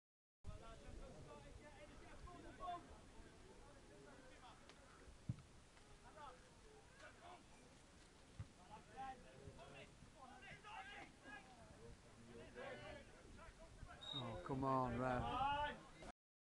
CCFA, SENIOR CUP 1st ROUND ,PENDEEN 4 V 0 ST STEPHEN
Match highlights, St Stephen player Chris Coad is fouled.